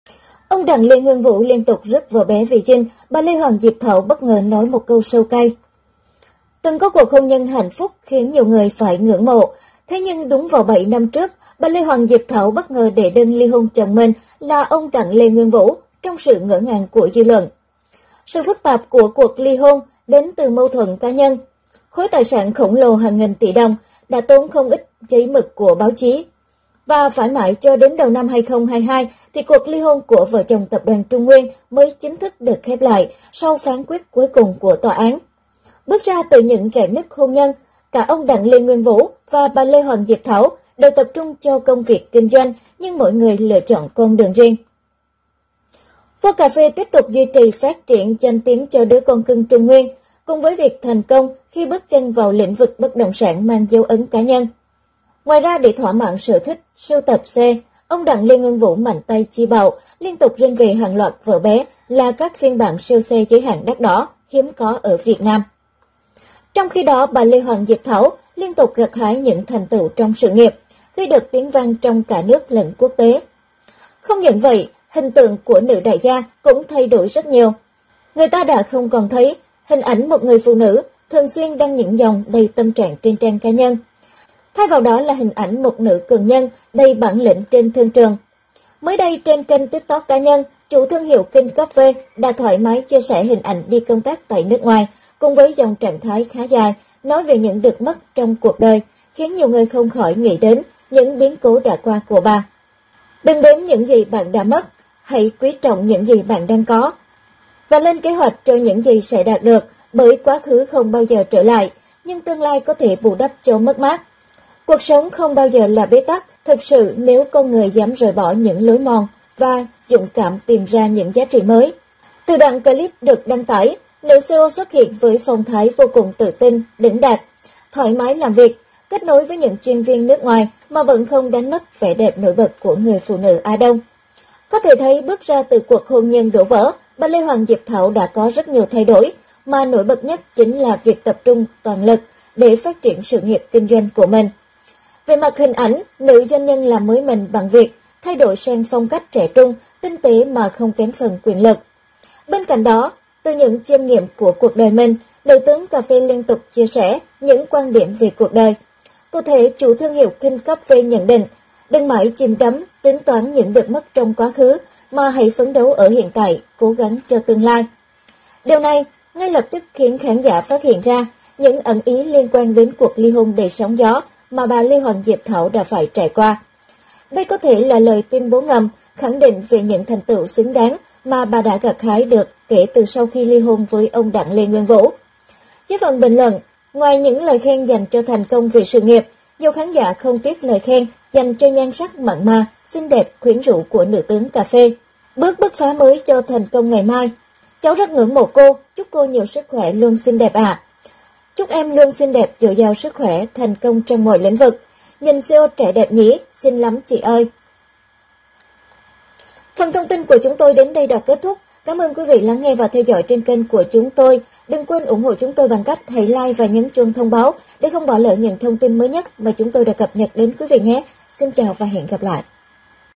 Ông Nguyên Vũ Rước 'Vợ Bé' Về Dinh Bà Diệp Thảo Bất Ngờ Làm Điều Này
Ông Đặng  Vũ Rước 'Vợ Bé' Về Dinh Bà  Diệp Thảo Bất Ngờ Làm Điều Này